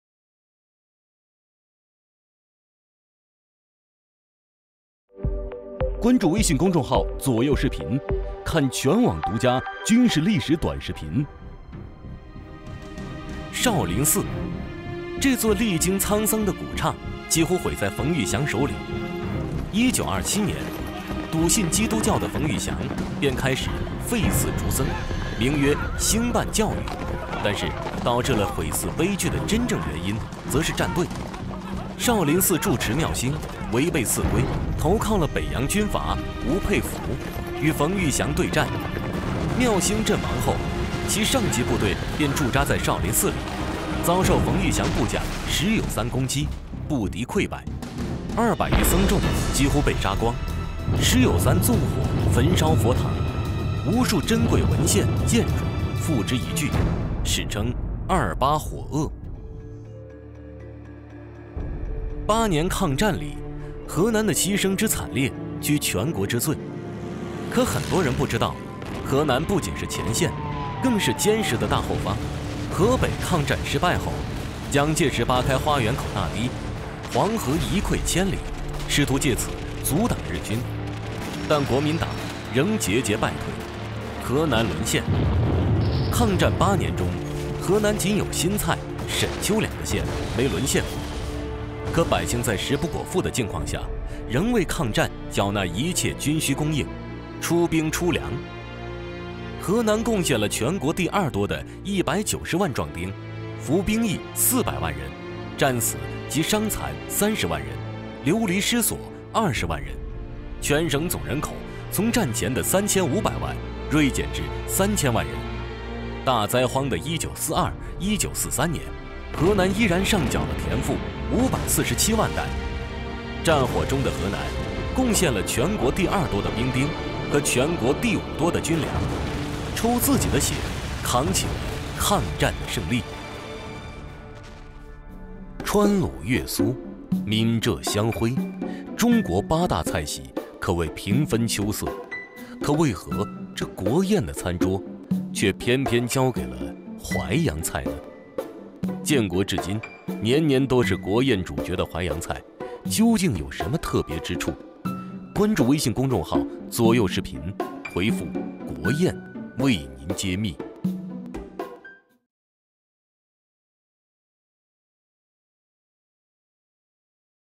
【左右视频】抗战期间这个省牺牲最大 饥荒时竟还出兵出粮
或者关注微信公众号“左右视频”（lrvideo）